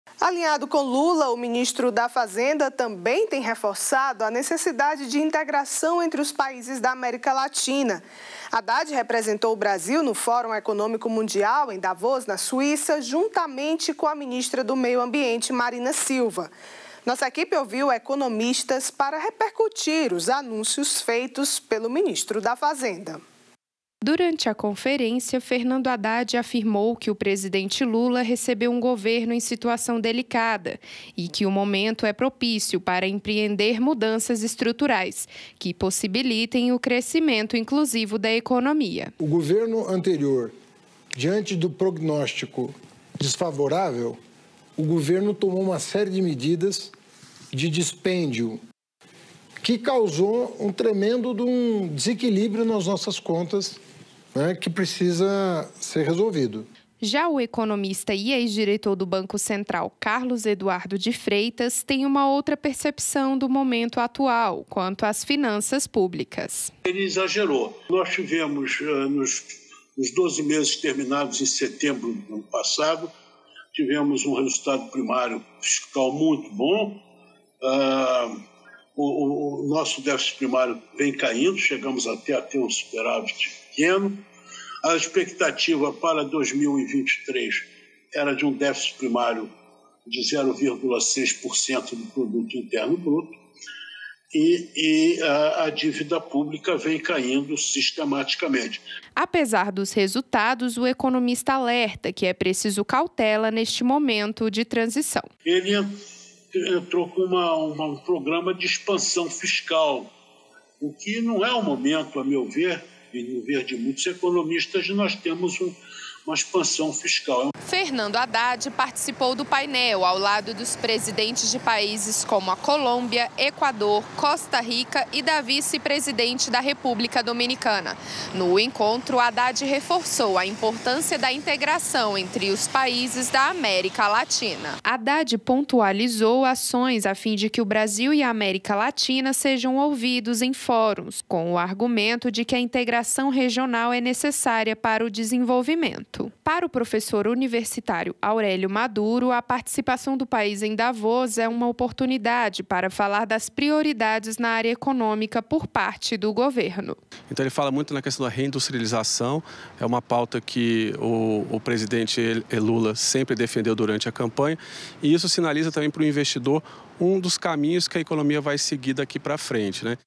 Economistas repercutem as falas do Ministro Fernando Haddad
Alinhado com Lula, o ministro da Fazenda também tem reforçado a necessidade de integração entre os países da América Latina. Haddad representou o Brasil no Fórum Econômico Mundial em Davos, na Suíça, juntamente com a ministra do Meio Ambiente, Marina Silva. Nossa equipe ouviu economistas para repercutir os anúncios feitos pelo ministro da Fazenda.

Reportagem de Pâmela Maria
Imagens de Sanny Alves e internet

Deus abençoe!